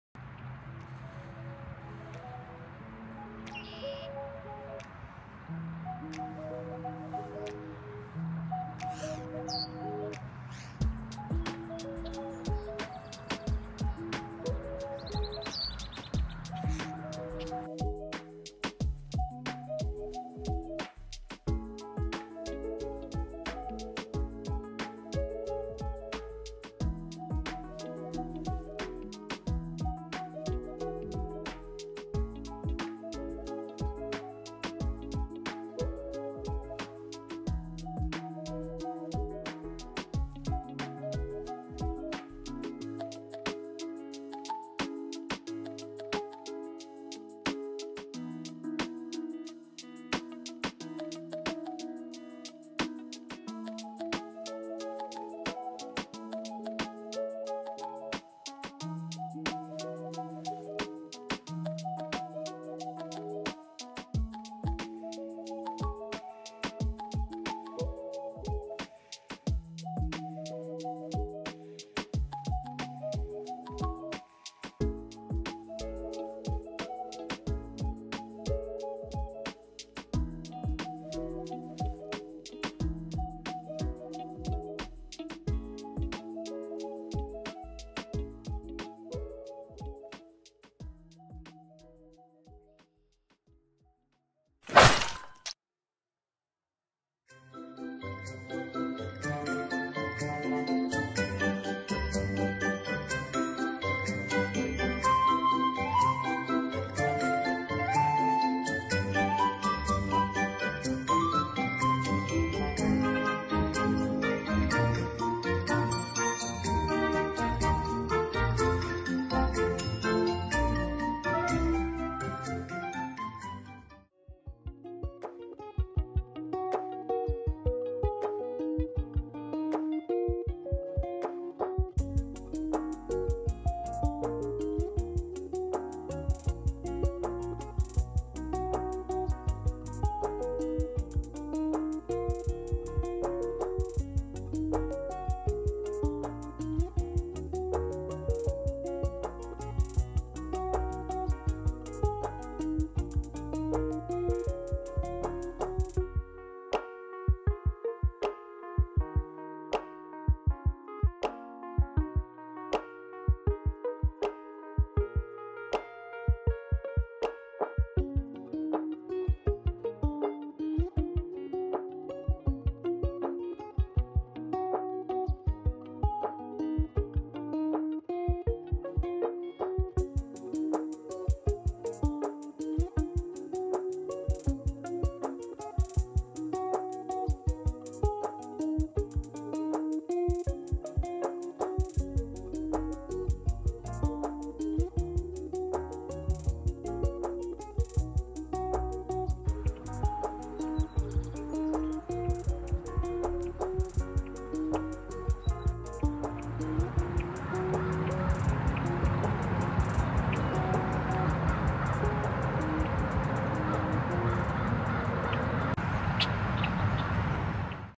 Great Blue Heron 大蓝鹭
Summer morning， Great Blue Heron hunting for food。夏日的清晨，一只大蓝鹭在池塘边捕食